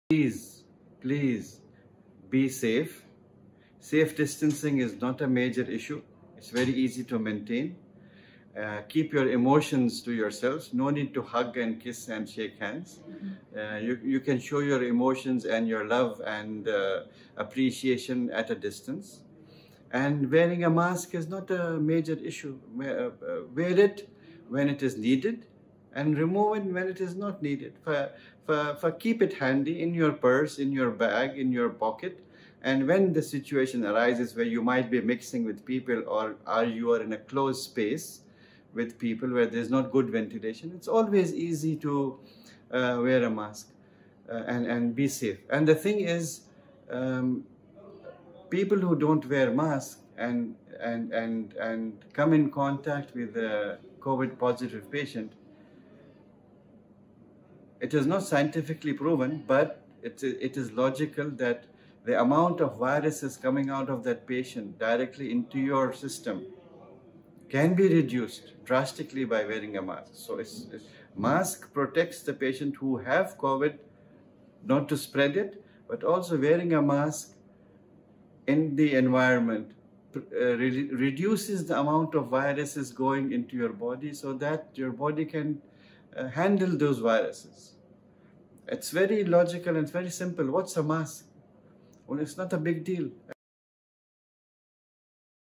Why a mask?
The Ministry of Health has called upon everyone  to take individual and social responsibility as well as comply with precautionary measures. Number one requirement is wearing a mask.